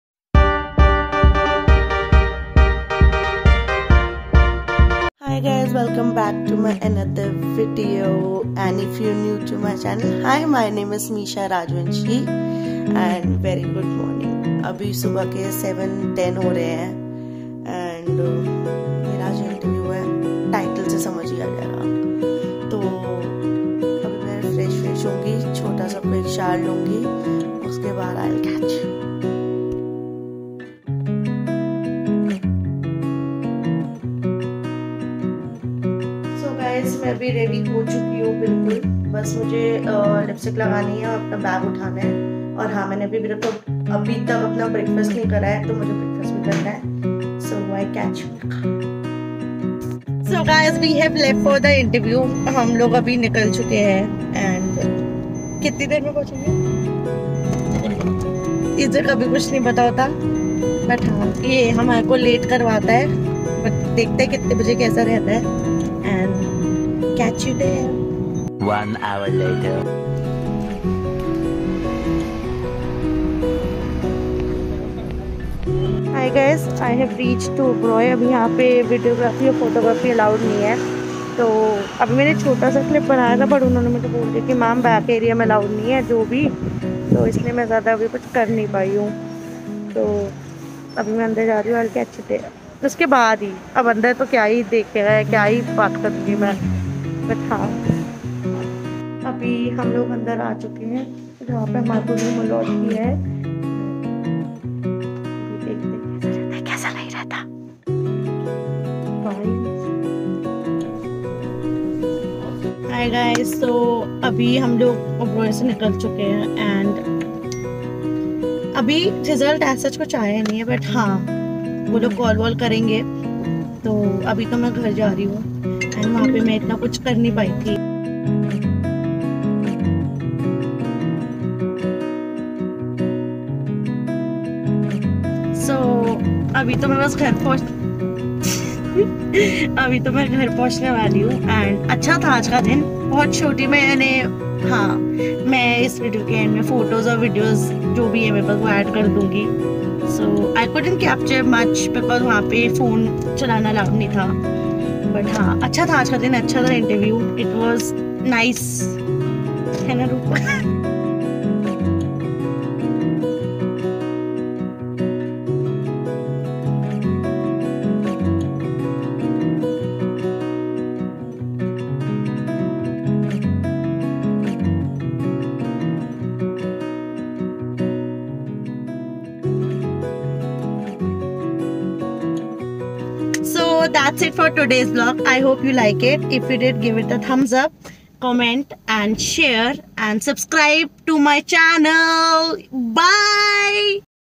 My first job interview | The Oberoi's gurgaon | Misha Rajvenshi
This short video is about my first interview at Oberoi's.
I hope u enjoy this short vlog 😇